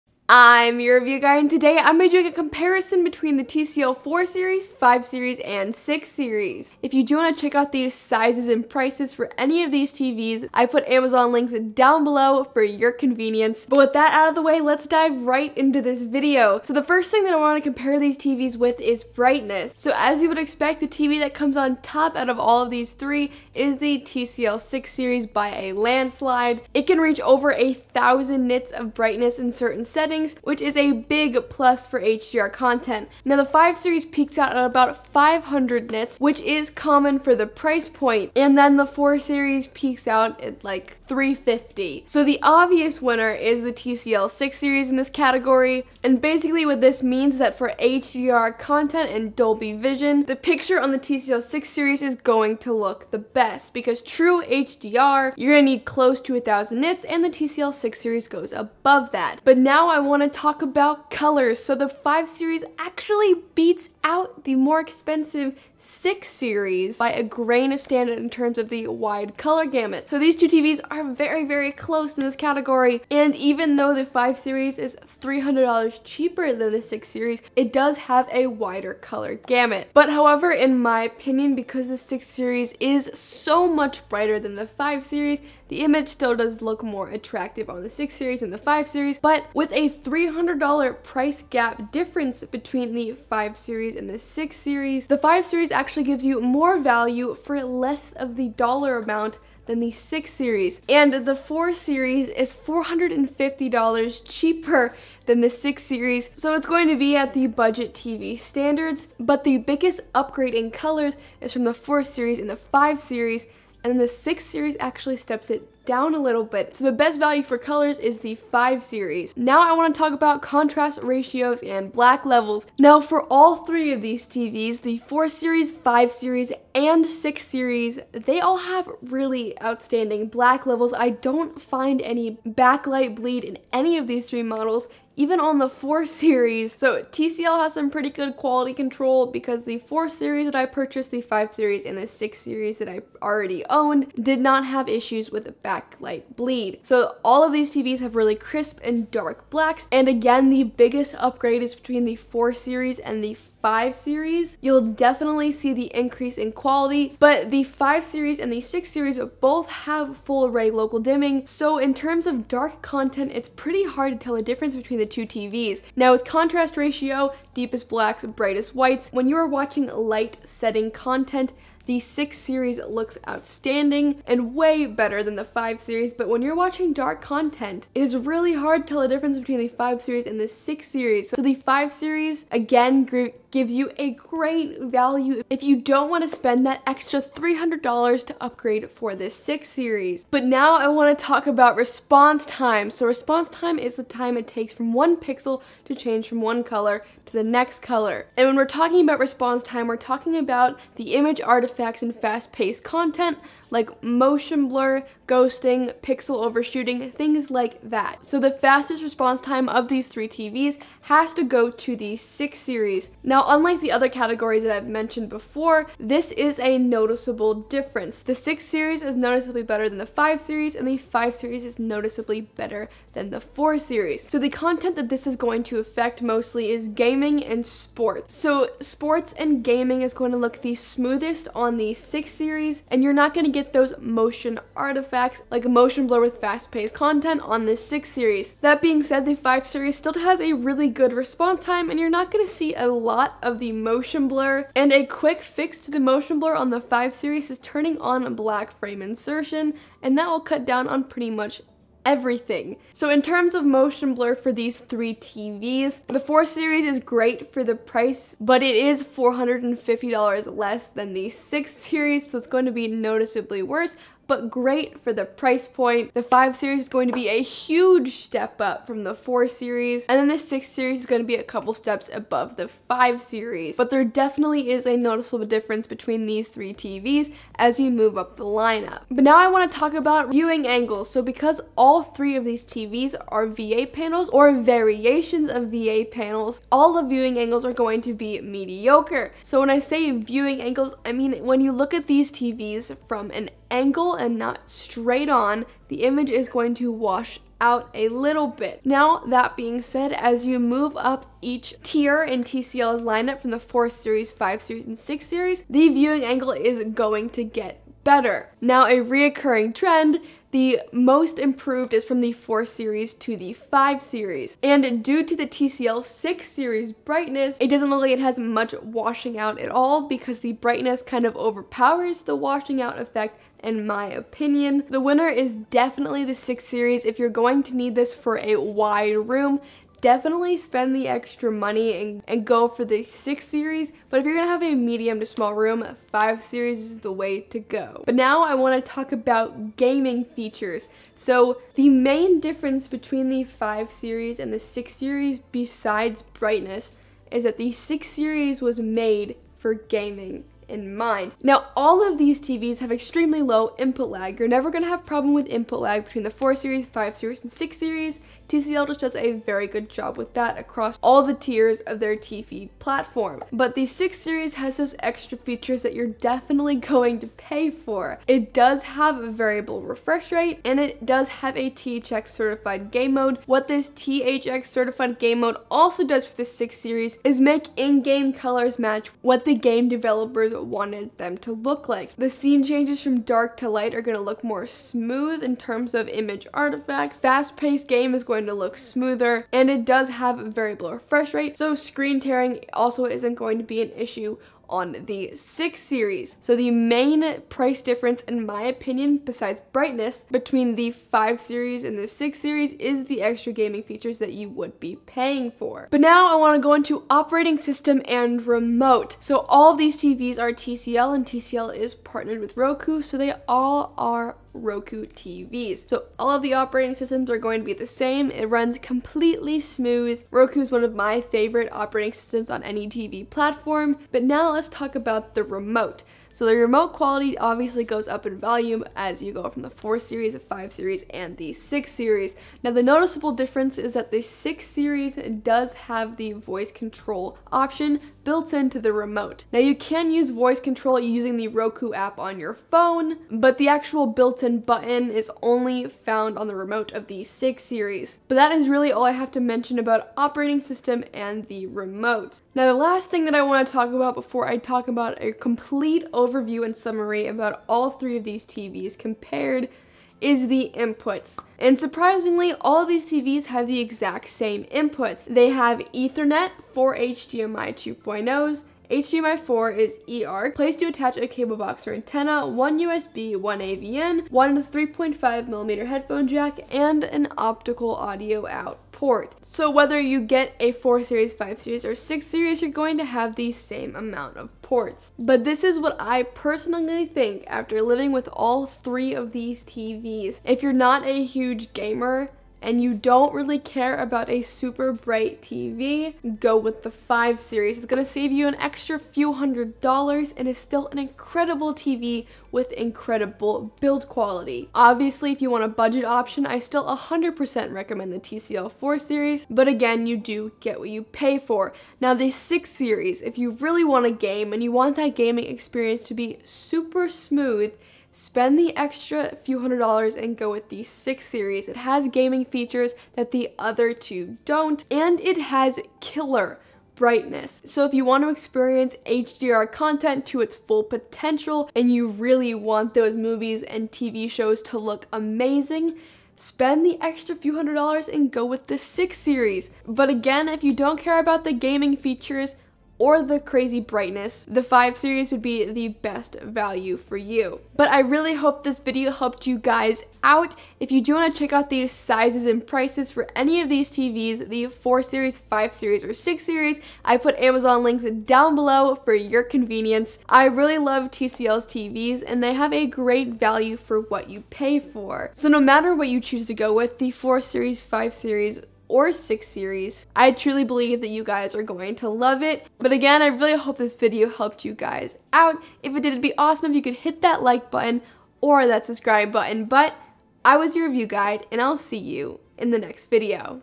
TCL 4 Series vs 5 Series vs 6 Series - TV Comparison
UK (Similar)

UK (similar)

UK (Similar)

Today, I am doing a video on TCL 4 Series vs 5 Series vs 6 Series - TV Comparison.  In this video, I do an in-depth comparison between the TCL 4 Series, TCL 5 Series, and TCL 6 Series.  I go over everything from black levels, brightness, sports, motion, and gaming features.  If you are looking for a tv comparison for TCL's lineup of TVs, I have you covered!  I highly recommend all of TCL's TVs and in this video, I try to help you find the best one for you; 4 series, 5 series, or 6 series.

Timestamps:
0:00 Intro
0:16 Brightness
0:59 Colors
1:59 Contrast/Black Levels
3:10 Response Time/Motion
4:39 Viewing Angles
5:38 Gaming Features
6:45 OS & Remotes
7:32 Inputs
8:05 Best for You?!